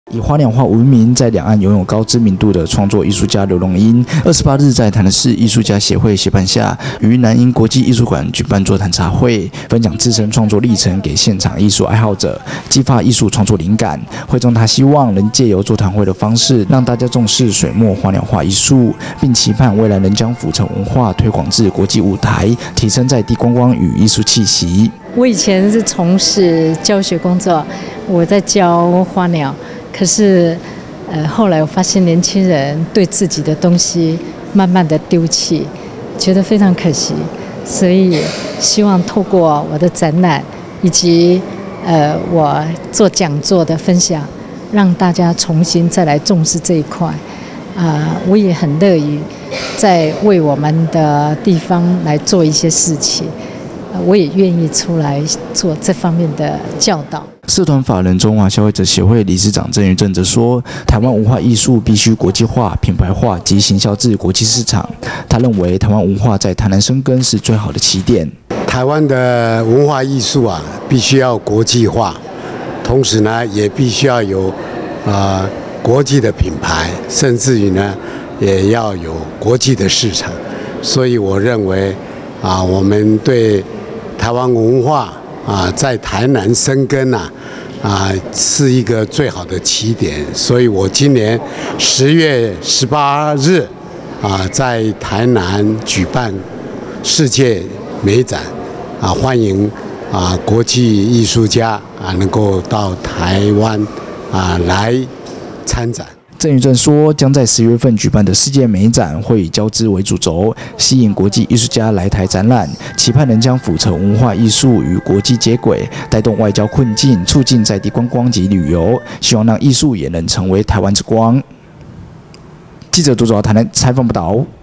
劉蓉鶯「水墨花鳥畫」藝術座談會 有別傳統開創新格局
【今日大話新聞／記者 杜哲華／臺南採訪報導】

以花鳥畫聞名，在兩岸擁有高知名度的創作藝術家劉蓉鶯，今（28）日於南瀛國際藝術館舉辦坐談茶會，分享自身創作歷程給現場藝術愛好者，帶領民眾賞析畫作，激發藝術創作靈感。劉蓉鶯表示，「人生是自己創造出來，因為繪畫使人生更豐富」，她希望能透過座談會的方式，讓大家更重視水墨花鳥畫藝術創作，也樂意將府城文化推廣至國際舞台，提升在地觀光與藝術氣息。（更詳細完整新聞請上《今日大話新聞》：